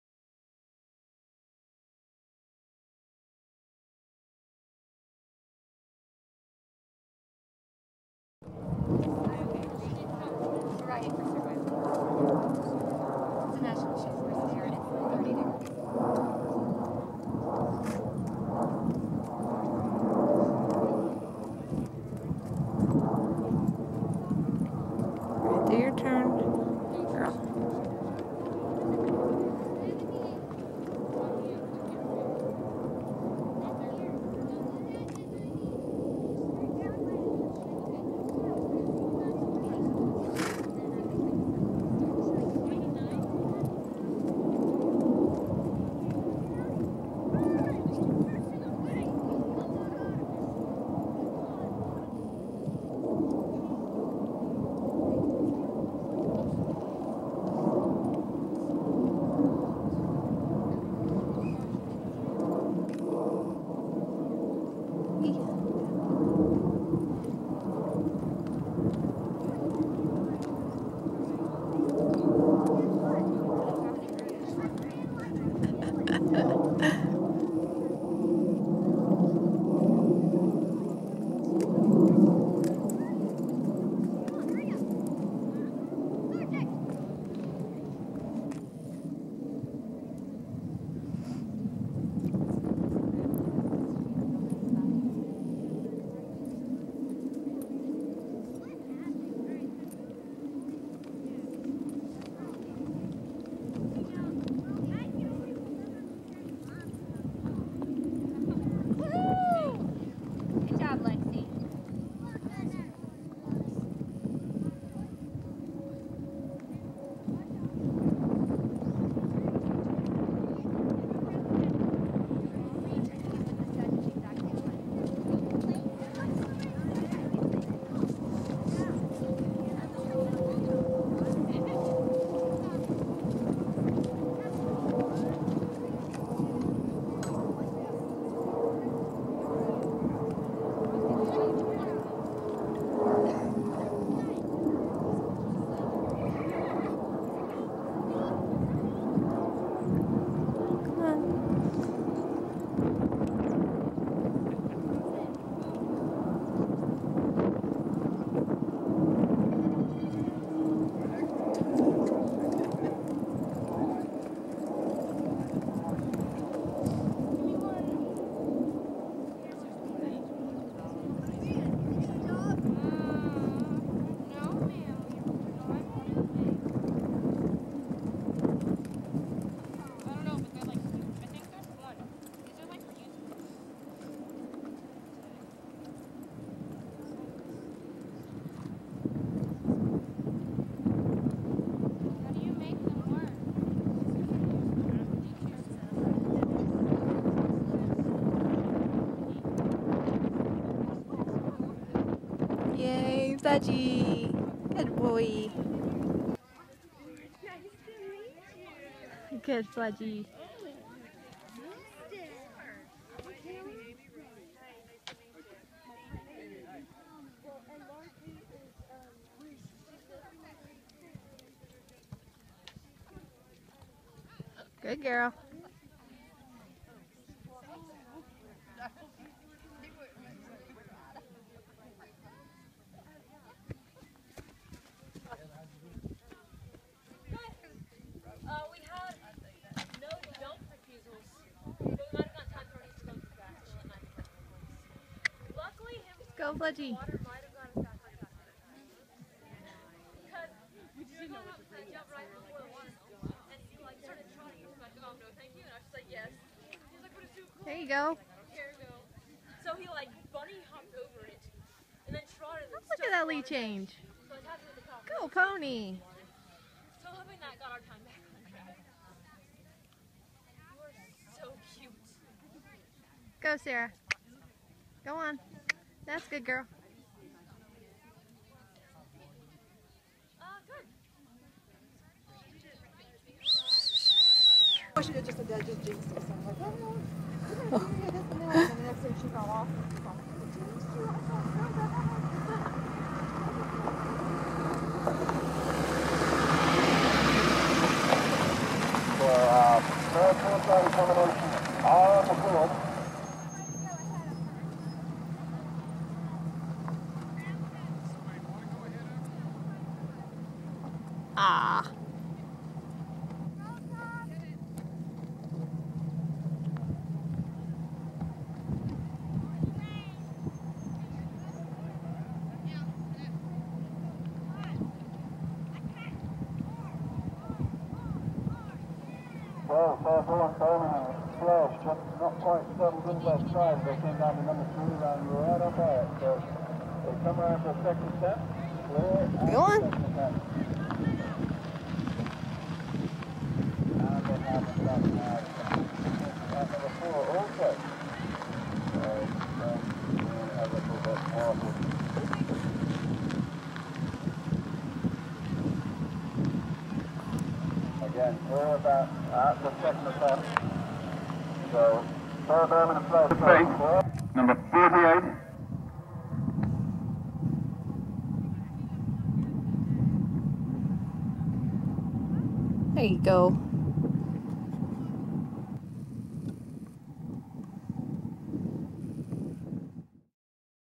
Sarah Bowman & Fledge: Chattahoochee Hills Schooling Show (November 2014)
PRESENTED BY ECOGOLD EQUESTRIAN TEXTILE ENGINEERING

Sarah Bowman goes schooling with Fledge (Of Extreme Mustang Makeover Fame at Chattahoochee Hills.  Show here is footage from all three phases: dressage, stadium jumping, and cross country